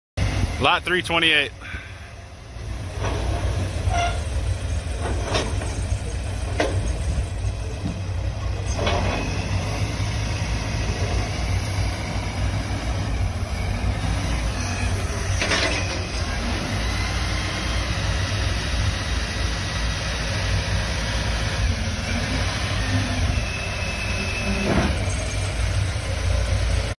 SKY TRAK 3606 For Sale
Skytrak 3606 Shooting Boom Forklift. SN 6646. OROPS. 4x4. 36 Foot Reach. 6600 LB Capacity. Cummins Diesel. 48 Inch Forks. Hour Meter Reads 1019 hours.